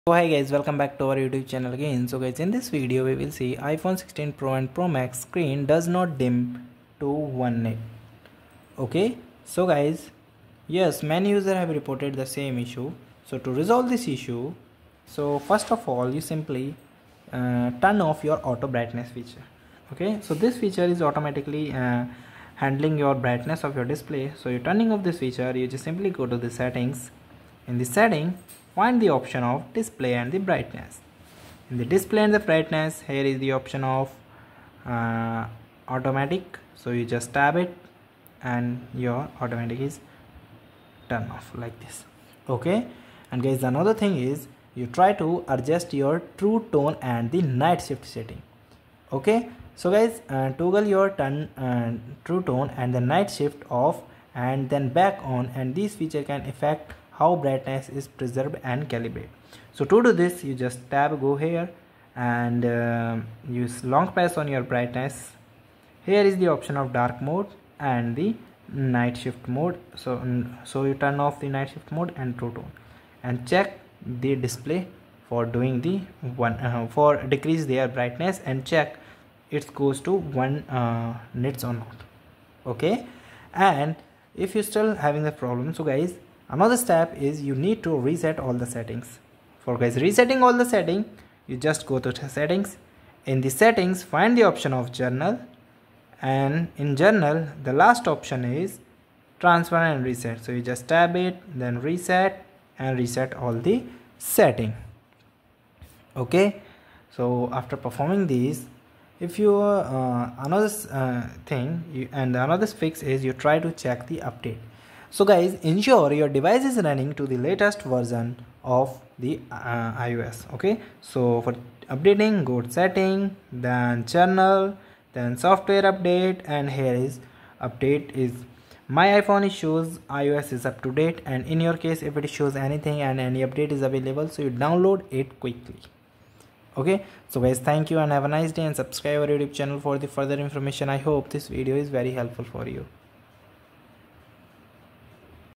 iPhone 16 Pro/16 Pro Max Screen Doesn’t Dim To 1 Nit: Here's How To Fix
In this informative video, we address a common issue faced by users of the iPhone 16 Pro and 16 Pro Max: the screen failing to dim to 1 nit.

0:15 Fix 1: Toggle Auto-Brightness:
0:43 Fix 2: Adjust True Tone And Night Shift Settings:
1:25 Fix 3: Reset All Settings:
1:52 Fix 4: Update iOS: